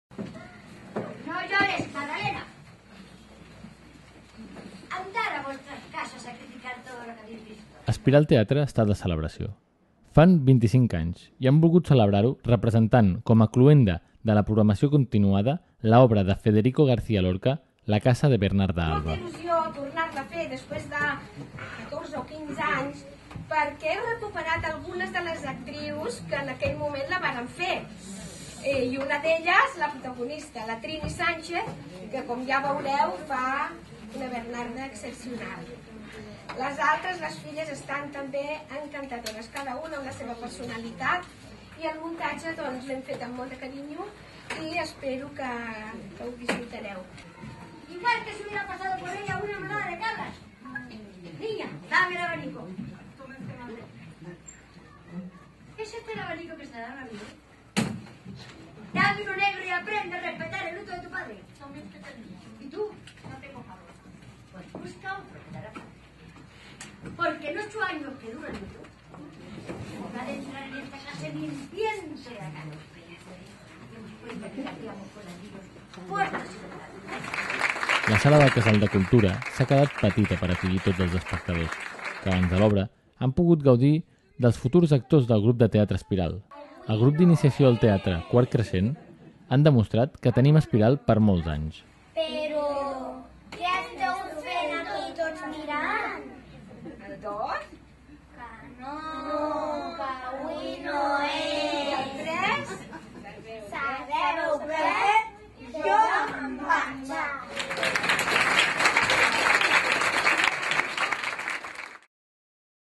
Teatre Espiral celebra els 25 anys amb "la casa de Bernarda Alba"
La casa de Bernarda Alba és l'obra escollida per a celebrar els 25 anys del grup de teatre Espiral, que a  més a presentat "espiral creixent" un grup de formació de futurs actors de la companyia.